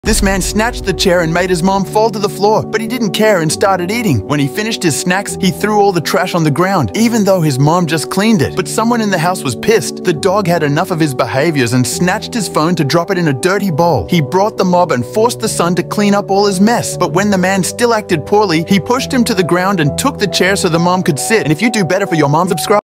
He thought he was right but regretted it ❤️
This man wanted to play with his mother and he took the chair and the mother fell and laughed a lot, they both played together and the man loved his mother very much and always took care of her. He tried to see what their dog will do so he threw the trash on the ground. and when the dog saw him doing that he rushed to take the man's phone and give him the broom to clean up the mess. The mother and the man laughed and petted thier loyal dog. Even when he was sitting, the dog came and pushed the man to make the mother sit.